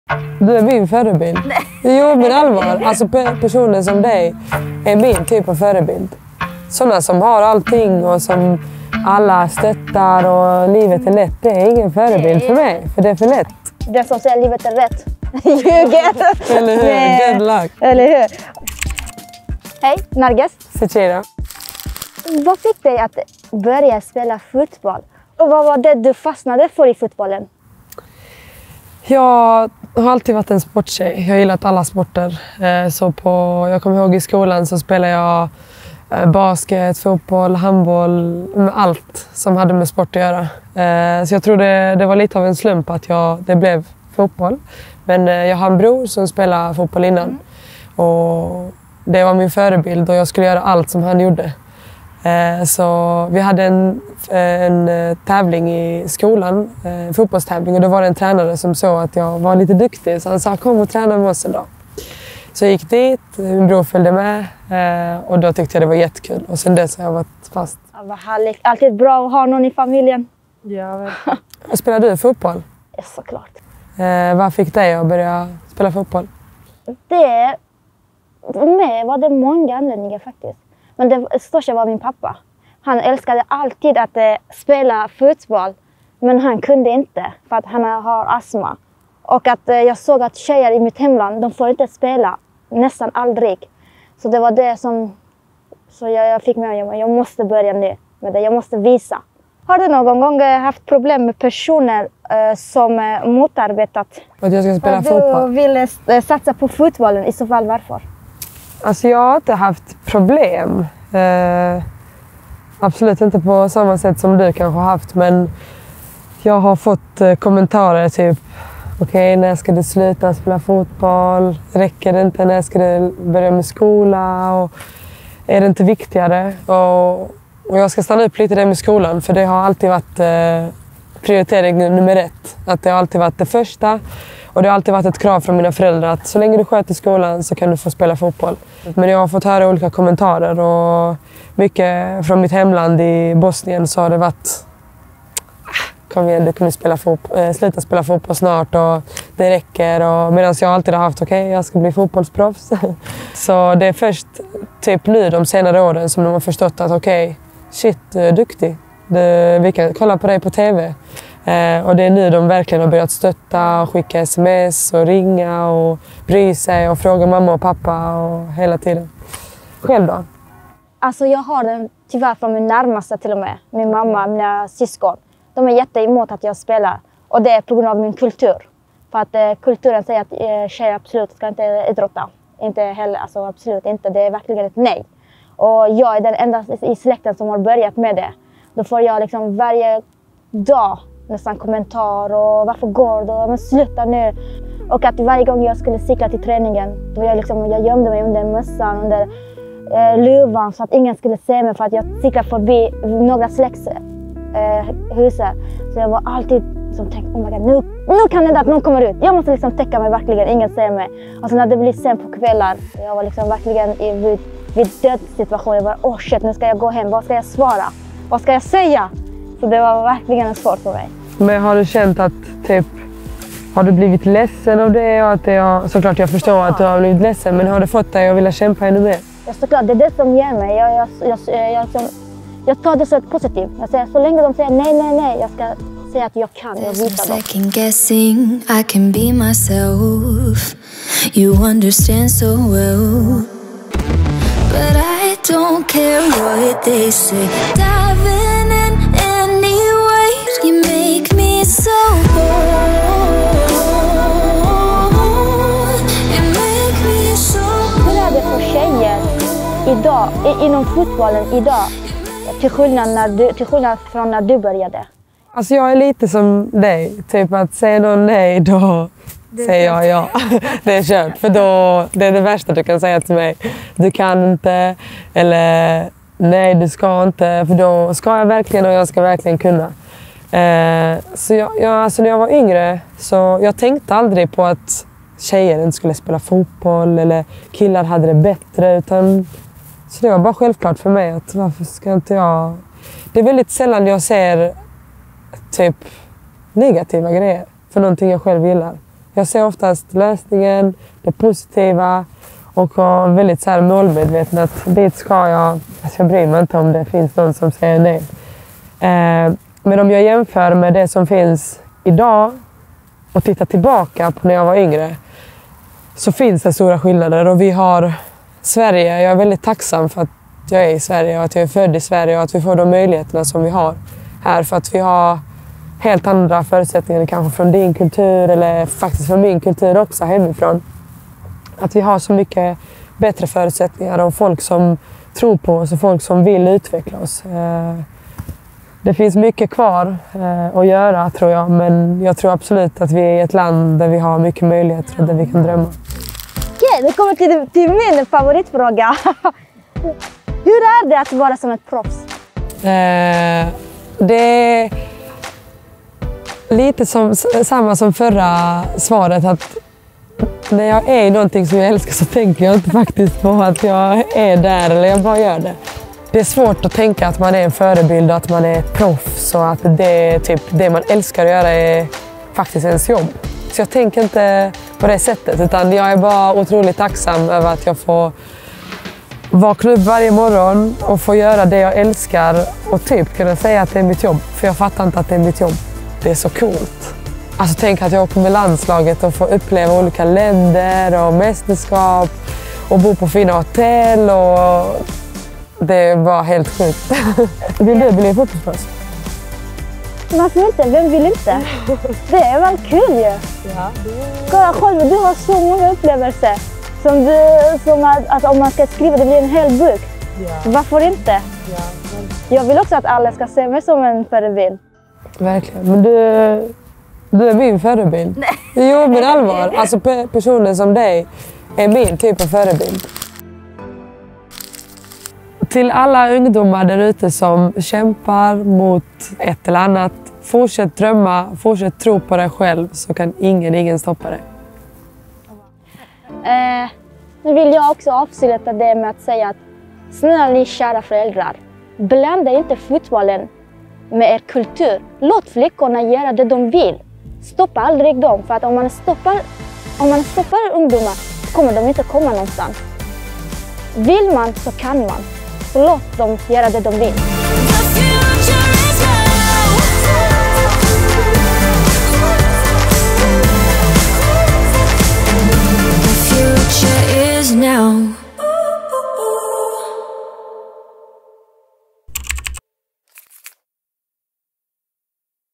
Fotboll för alla - afghansk kvinnokamp och proffsliv
Möt två starka, karismatiska fotbollstjejer som gör det de brinner för och tror på! FC Rosengårds och svenska landslagets Zecira Musovic intervjuas av Kvarnbys Narges Rahmani. Två målvakter med rötter i Bosnien och Afghanistan och med stor passion för fotbollen, pratar idrottsdrömmar, kvinnosyn och proffsliv.